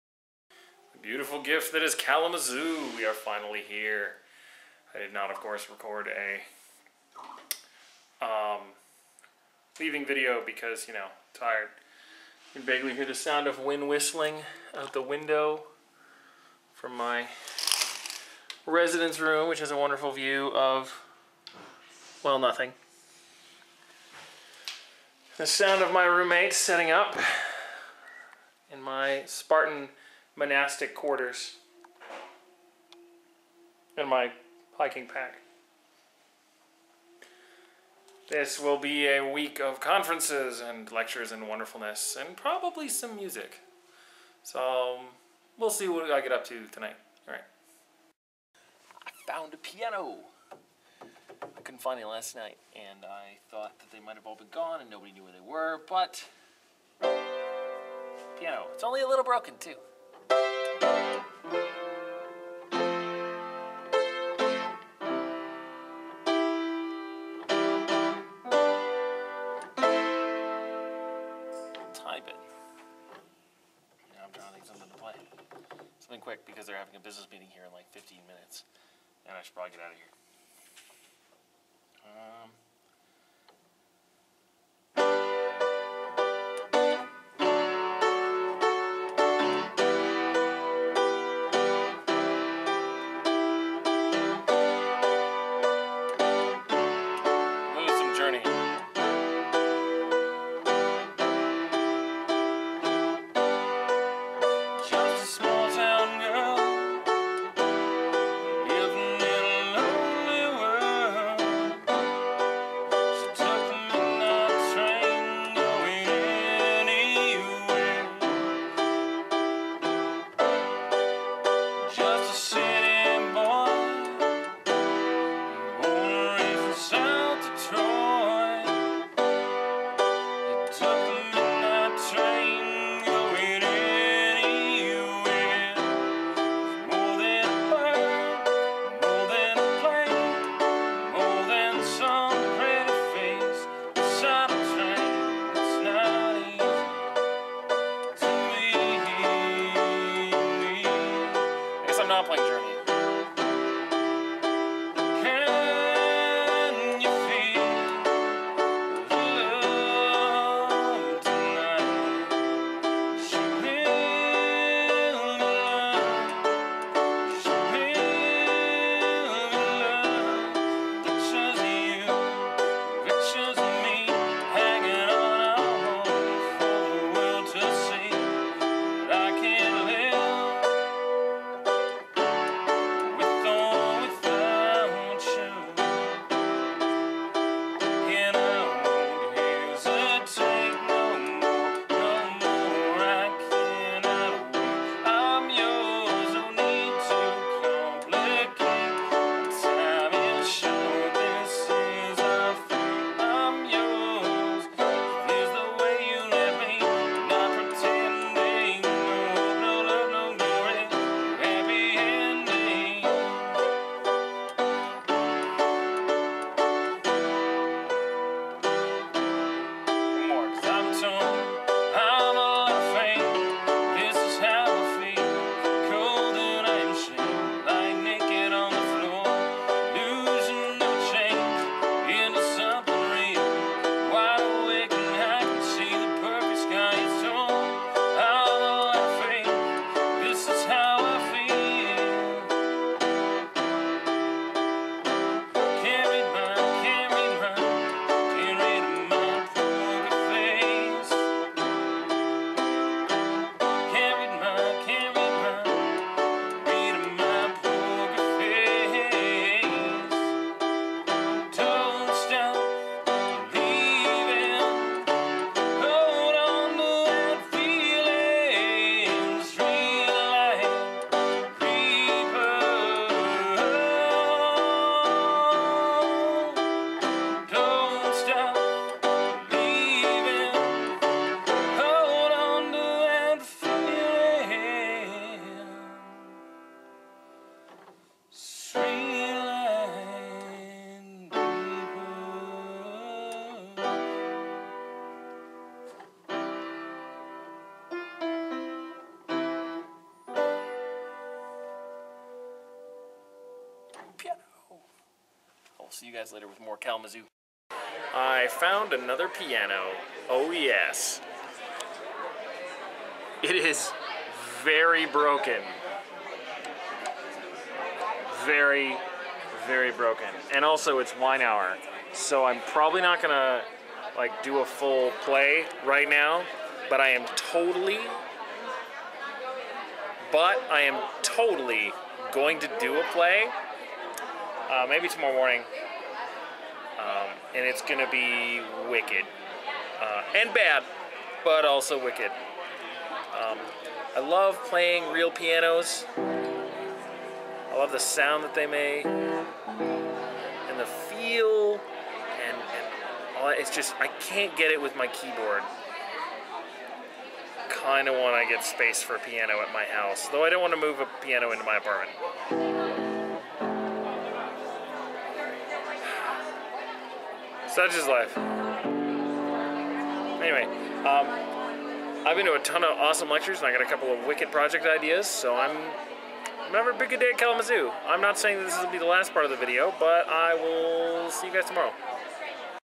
Kalamazoo Day 1
I'm at the International Congress on Medieval Studies in Kalamazoo, Michigan. Day 1: I make music noises.